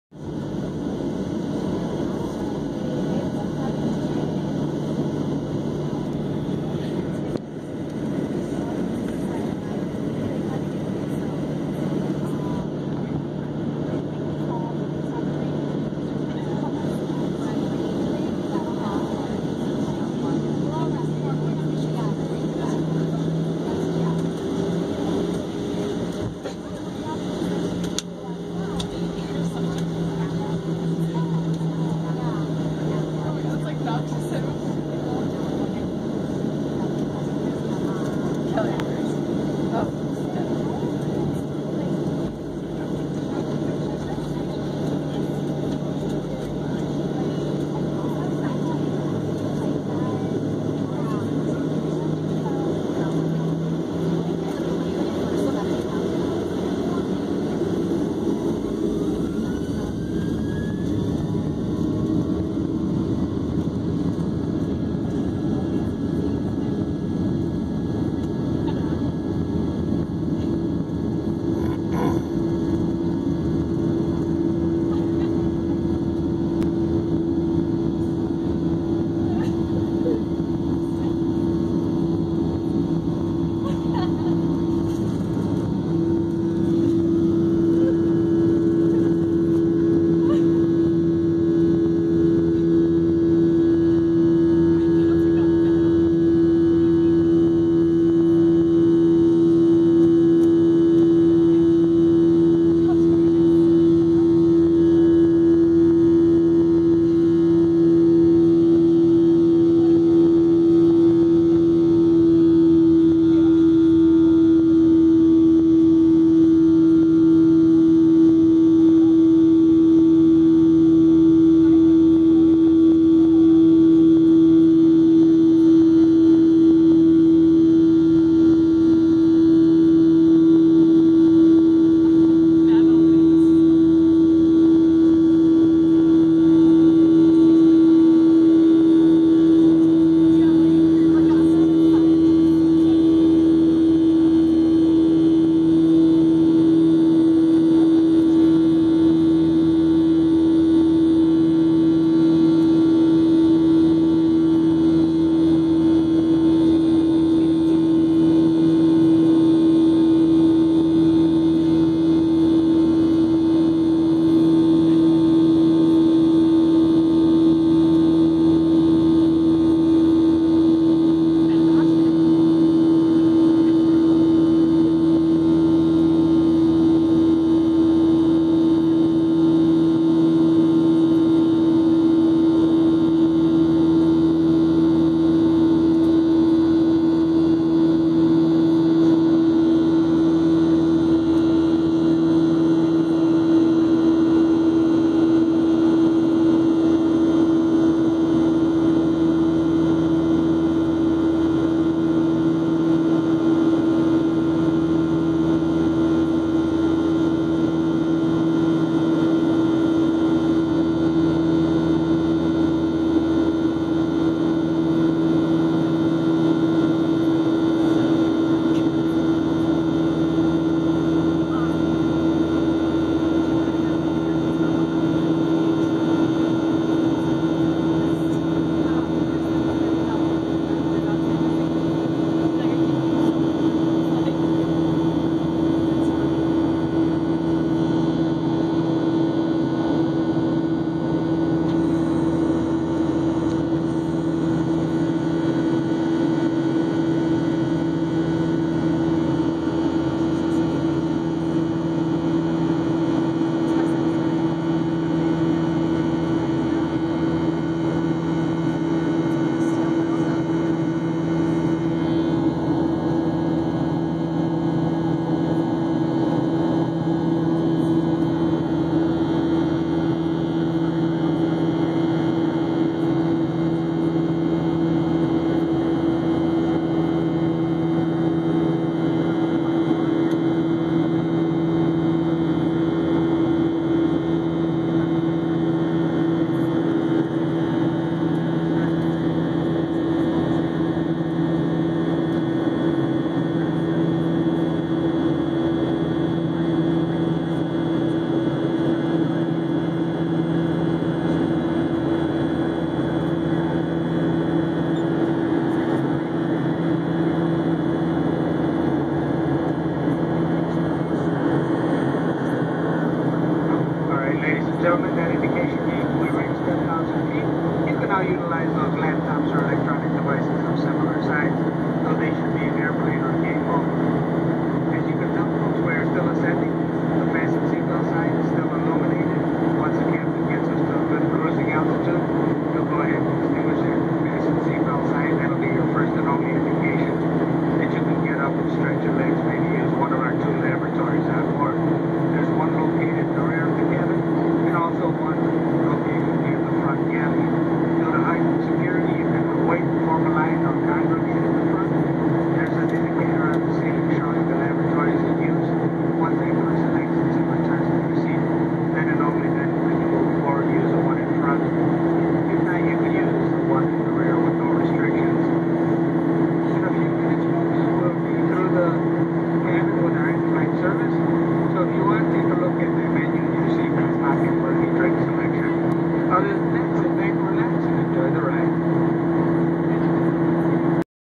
Whats it like to fly? Take off.
Whats it like to fly - 737-700 taking off from Chicago Midway airport.